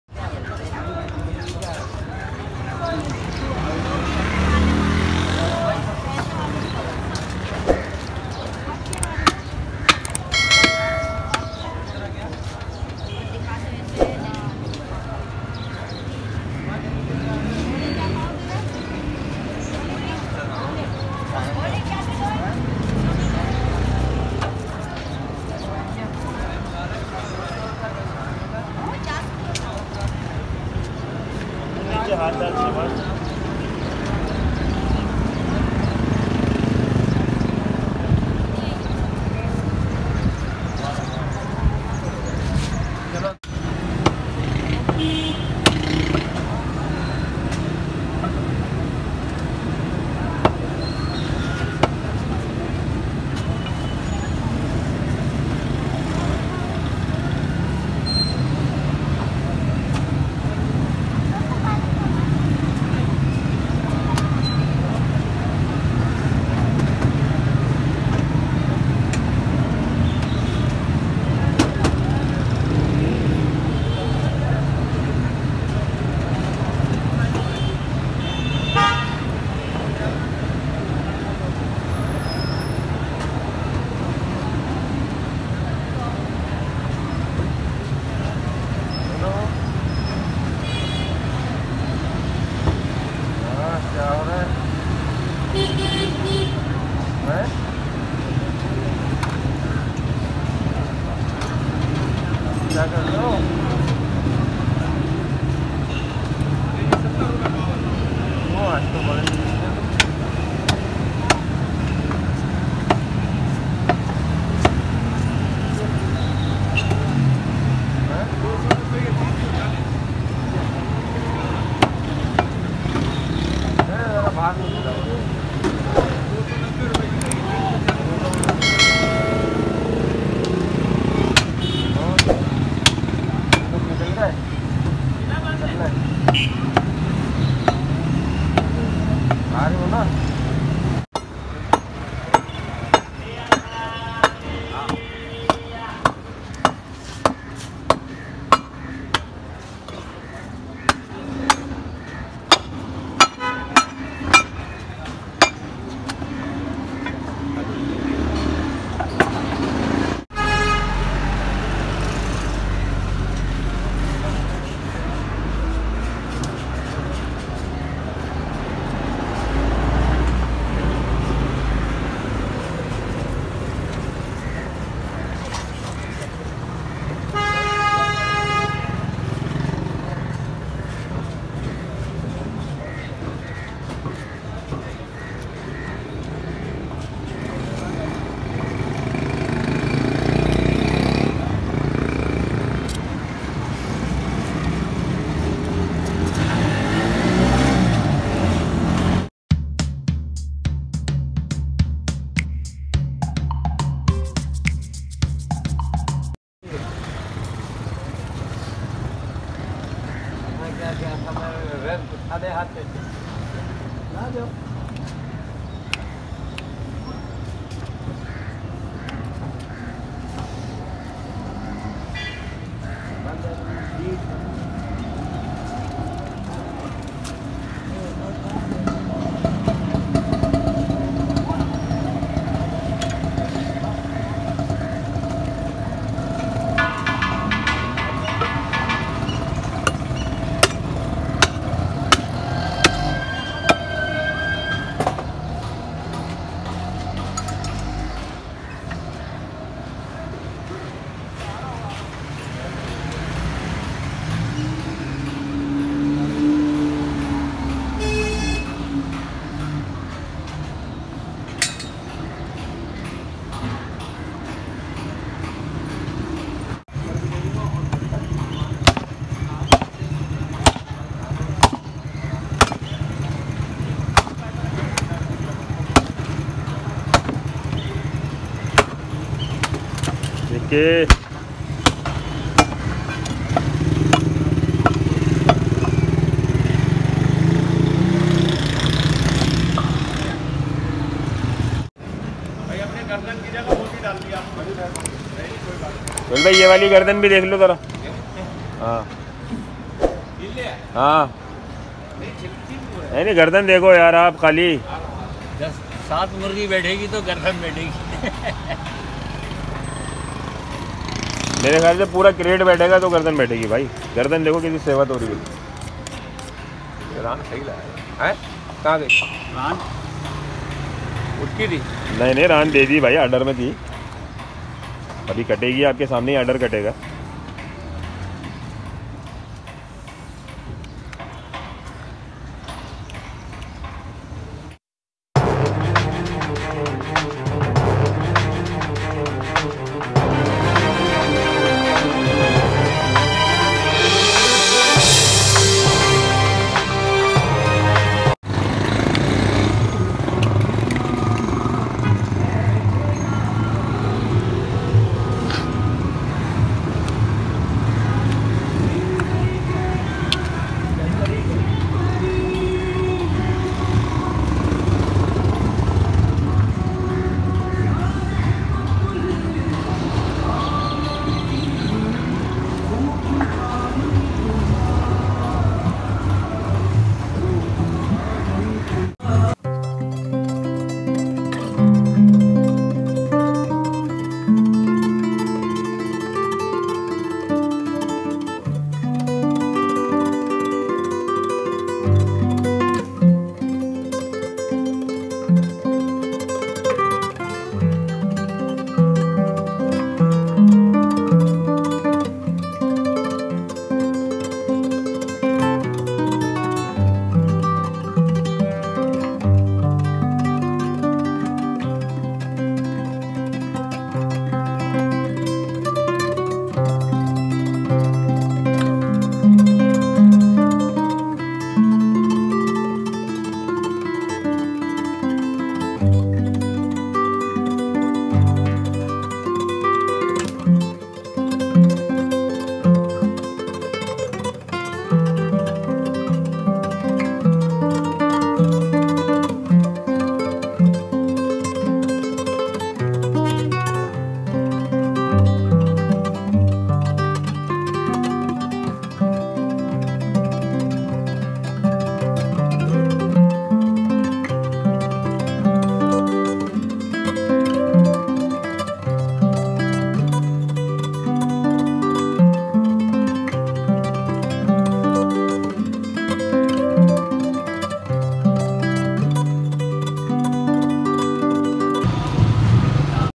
Meat Cuttint | Cow Meat Cutting By Expert Butcher In Karachi Pakistan
Meat Cutting | Cow Meat Cutting By Expert Butcher In Karachi Pakistan ||
Hello Friends.
WelCome To A Best Beef Meat Cutting And Selling . You Will See Exclusive Meat Cutting And Processing By A Professional Butcher In A Meat Shop.